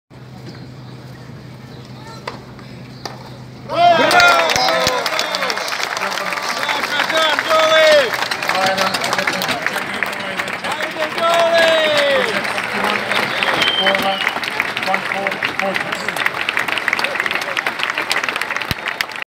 Match point Novak Djokovic Alexander Zverev 2:1 Belgrade Adria Open 202020200614 172106
Hot Shot Novak Djokovic Alexander Zverev 2:1 Belgrade Adria Open